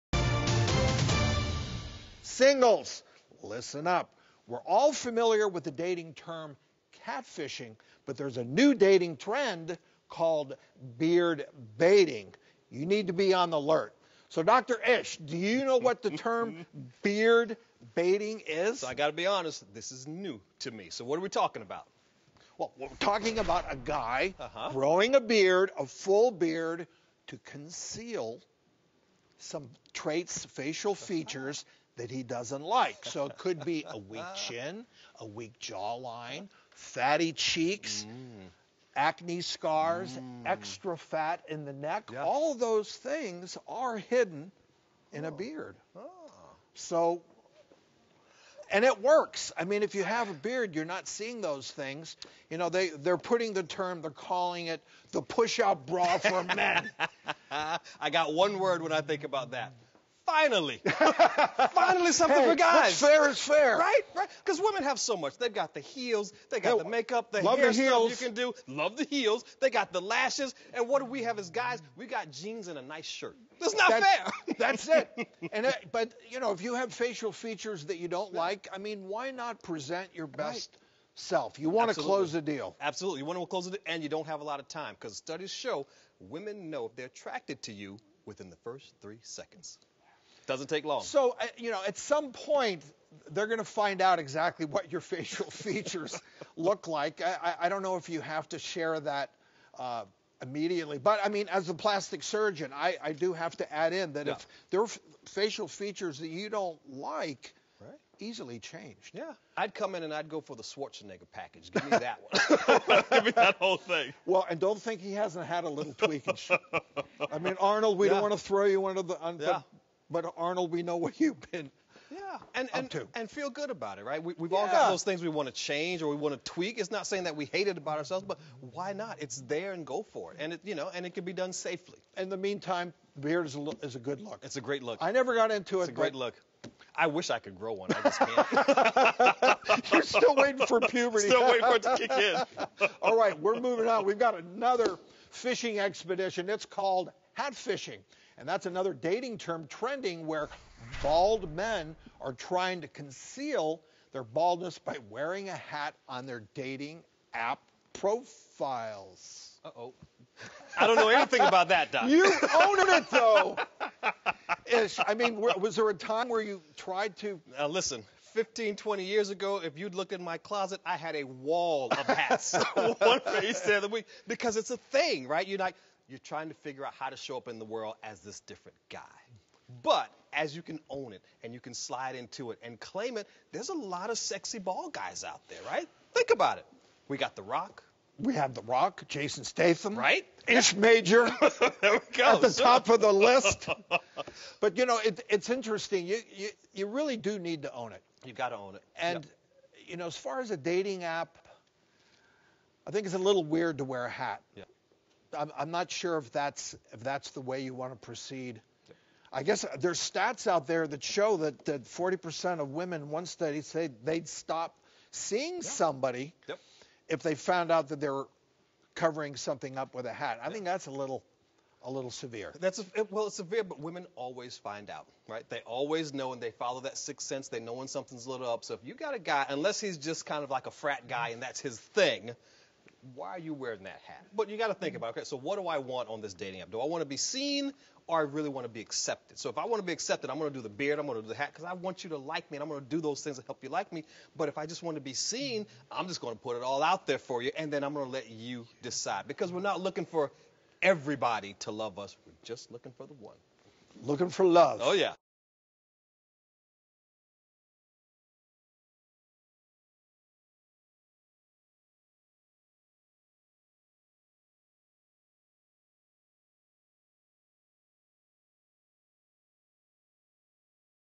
Daters Beware of Beard Baiting and Hatfishing!
When trying to assess attractiveness from a photo, beware of beard baiting and hatfishing. Beards can conceal facial features like a weak chin, fatty cheeks, acne scars, and even extra fat in the neck. Also, before you swipe right beware of hatfishing! This trend has bald men trying to conceal their baldness on their dating app profiles.